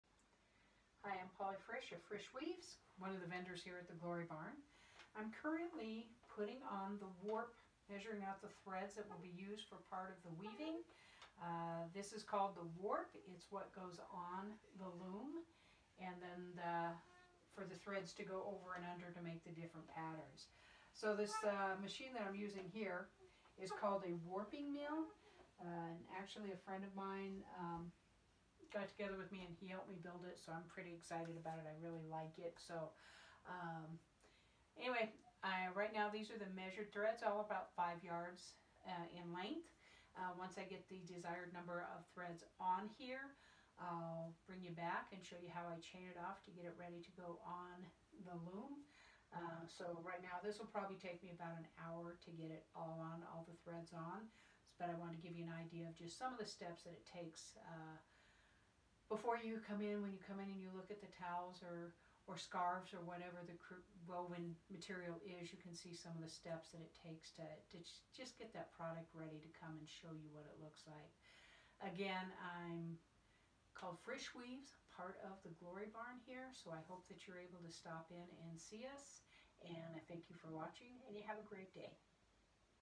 How to put a warp on the warping mill
Demonstration of putting a warp on a warping mill as well as an explanation of why.